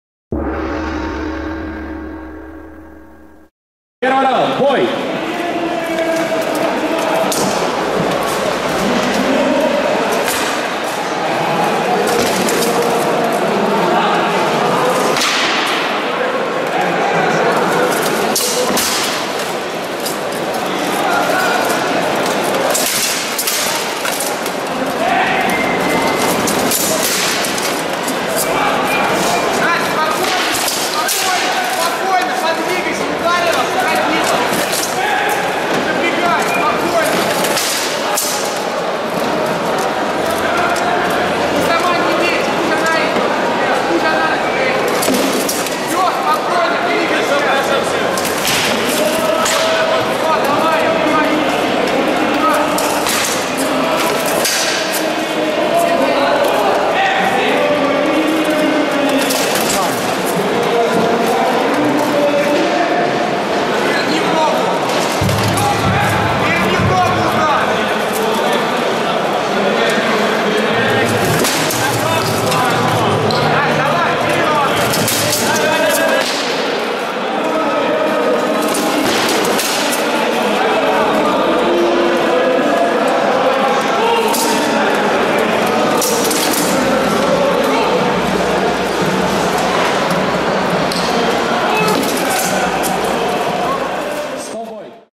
Рекон 2018 Щит Жен 1fight Сафонова vs Мещерекова
23-24 февраля в г. Санкт-Петербург отгремел РЕКОН-2018 – один из самых масштабных фестивалей военно-исторического движения, объединивший на своих площадках реконструкторов всех эпох со всей России. Во второй год проведения РЕКОН стал, пожалуй, главным системообразующим